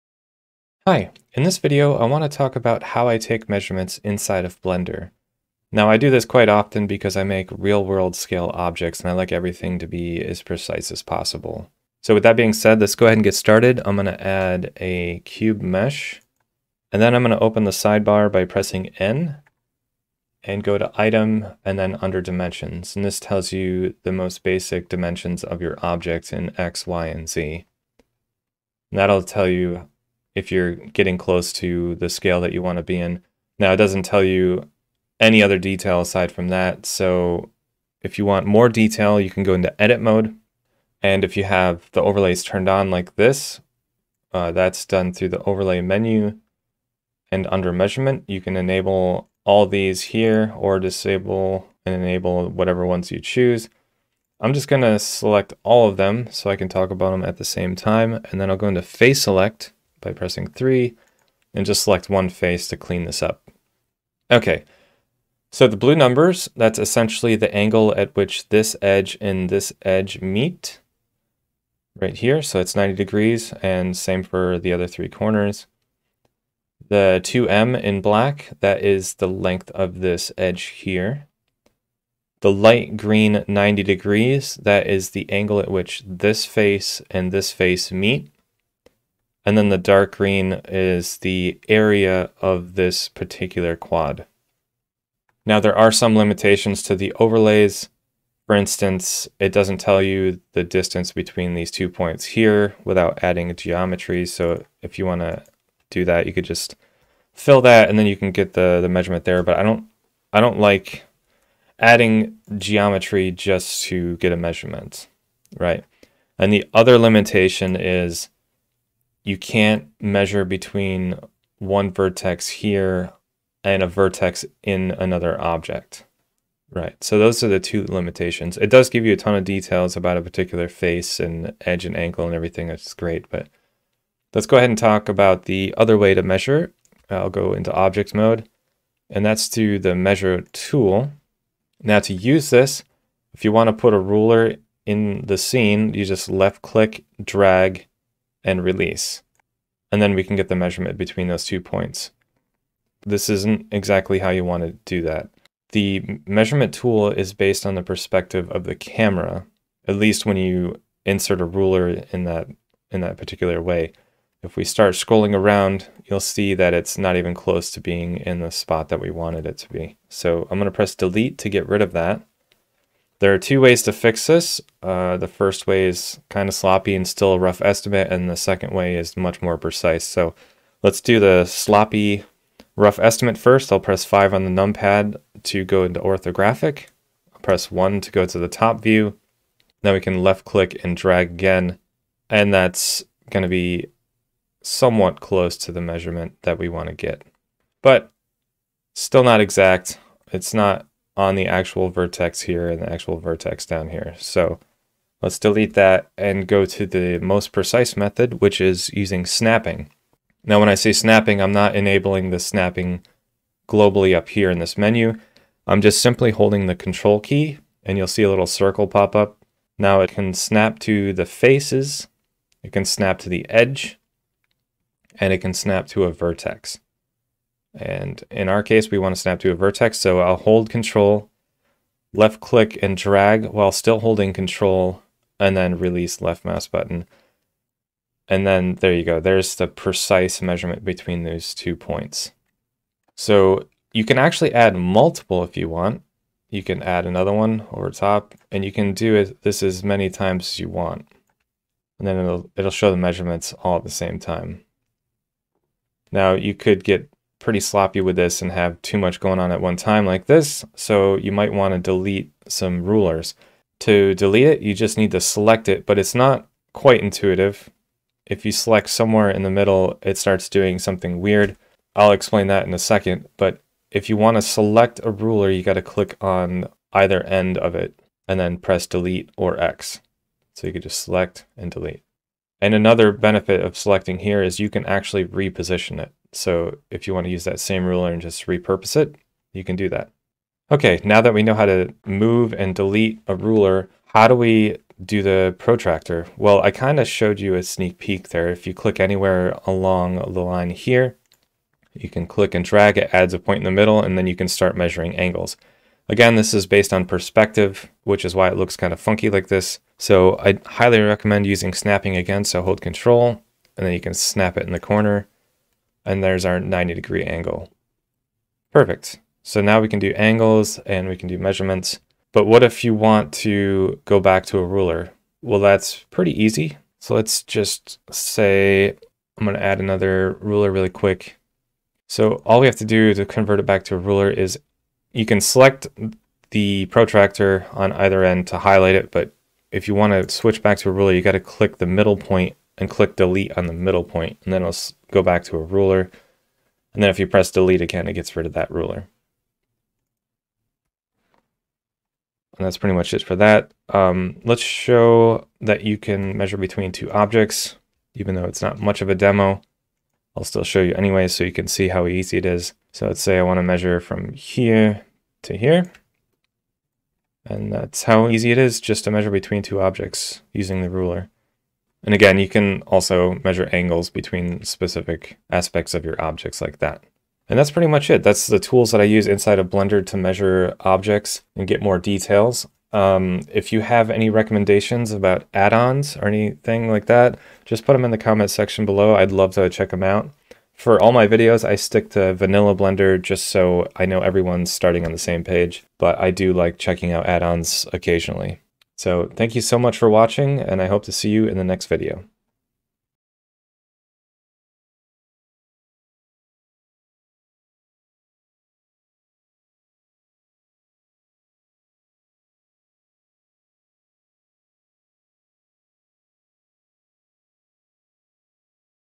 Never Estimate Again: Accurate Object Measurement in Blender
Learn how to easily measure objects in Blender 3.0.1. I cover the three most common methods of measuring objects: object dimensions in the sidebar, measurement overlays, and the measure tool.

TL;DW (Too Long; Didn't Watch)

Chapters:
0:00 - Intro
0:16 - Basic Measurements
0:38 - Overlay Measurements
2:16 - Measure Tool